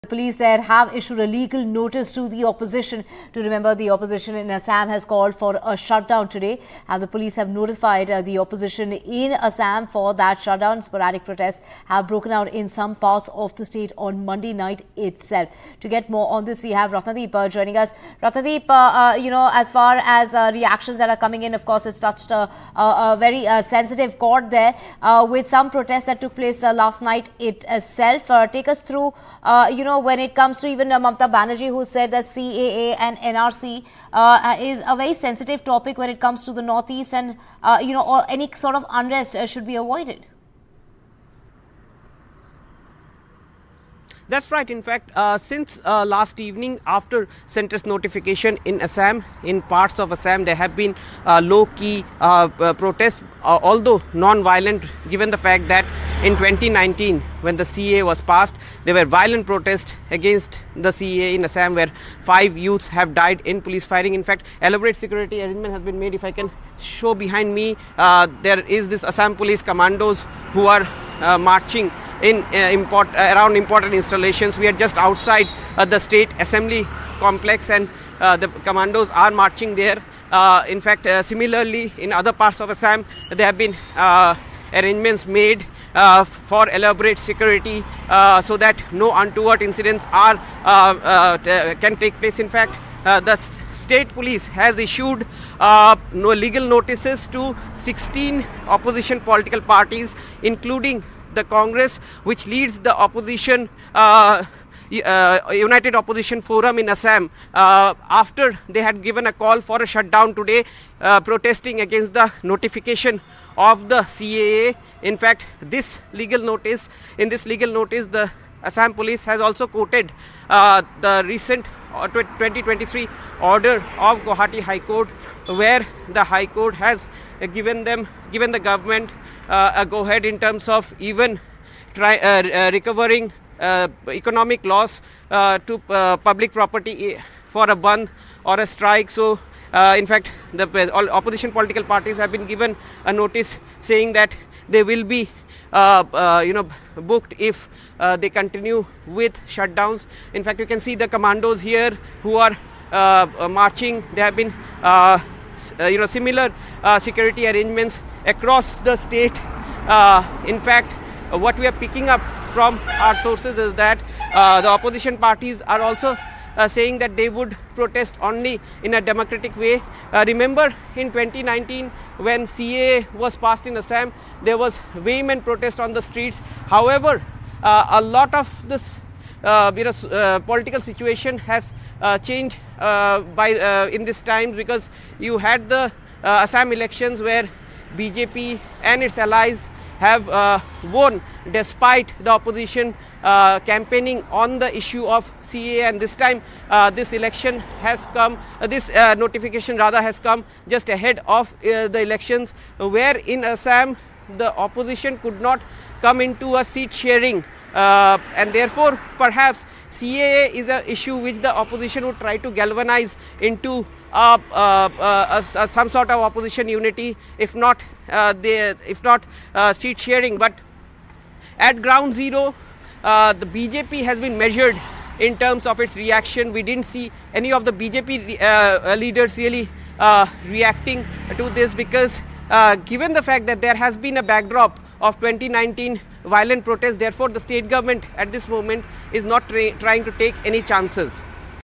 CAA Protest In Assam | Assam Opposition's Shutdown Call As Citizenship Law CAA Implemented
Protests erupted across Assam with the Opposition parties slamming the Centre's decision to implement the contentious Citizenship (Amendment) Act, 2019 (CAA), paving the way for granting citizenship to non-Muslim migrants from Pakistan, Bangladesh and Afghanistan.